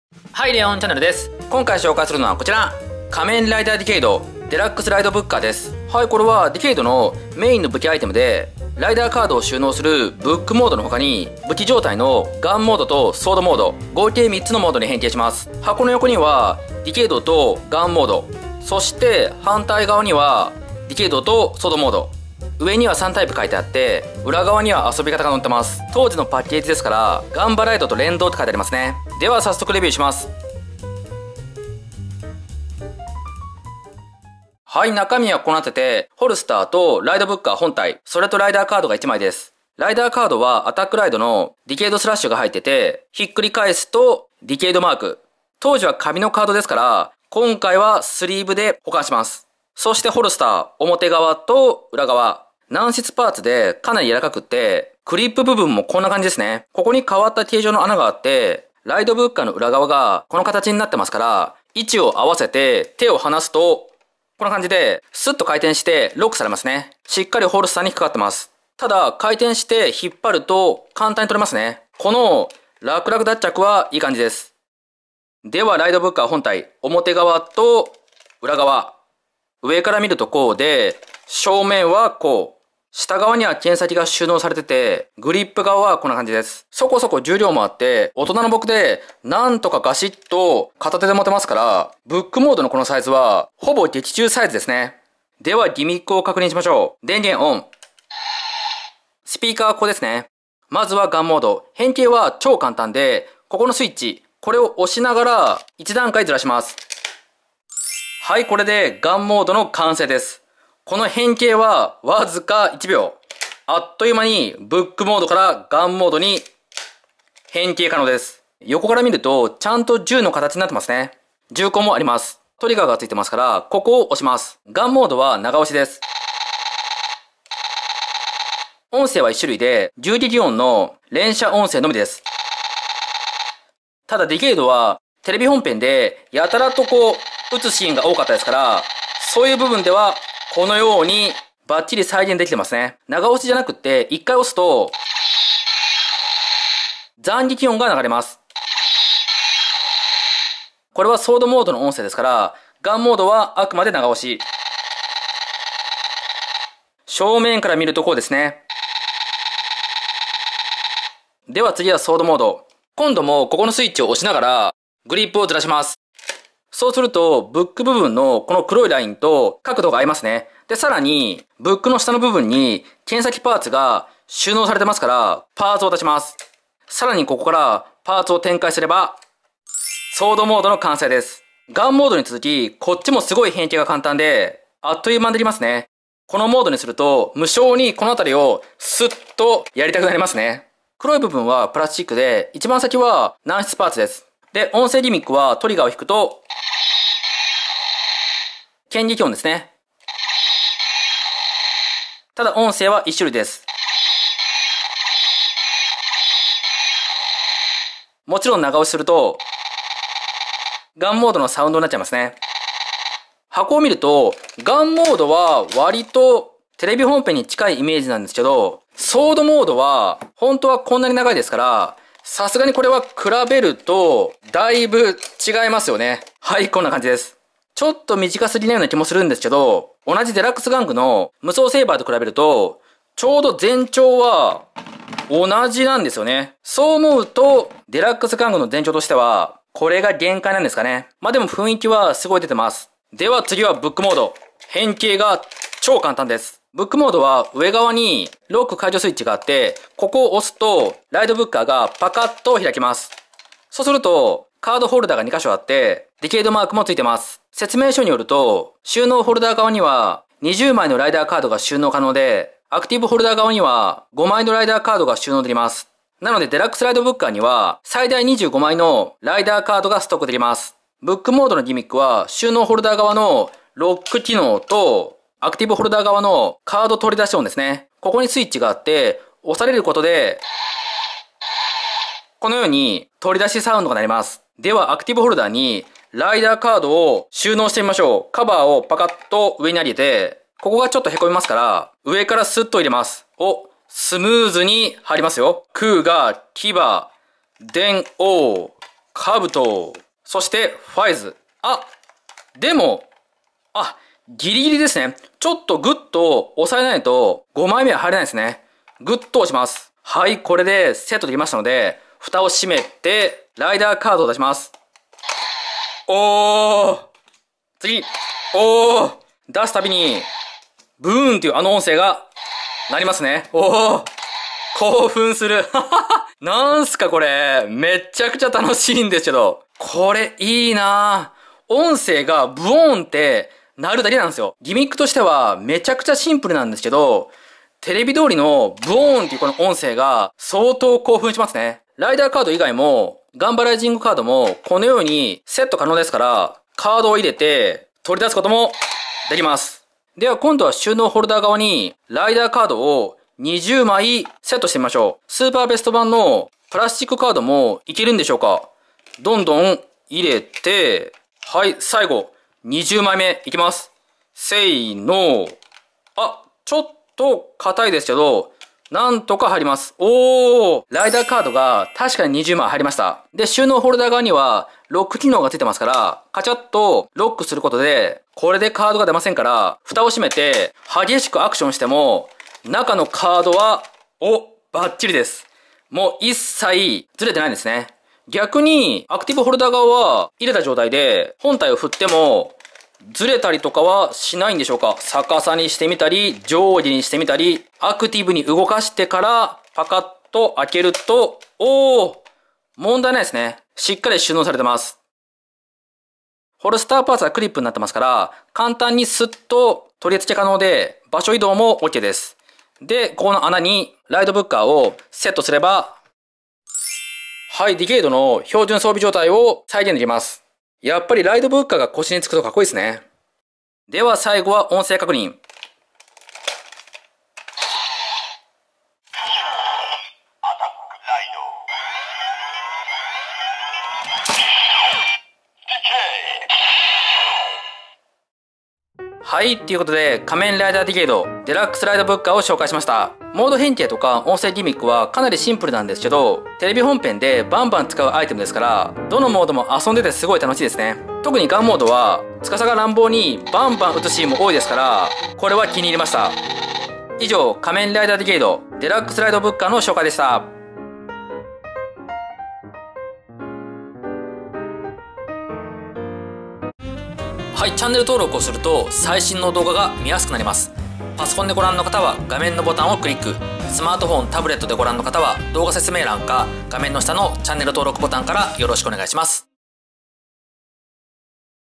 ライダーカード25枚収納 & 変形！DXライドブッカー 仮面ライダーディケイド レビュー！ブックモード・ガンモード・ソードモード3タイプ変形 DXディケイドライバーでライダーカード音声確認
DXケータッチ
※近日公開予定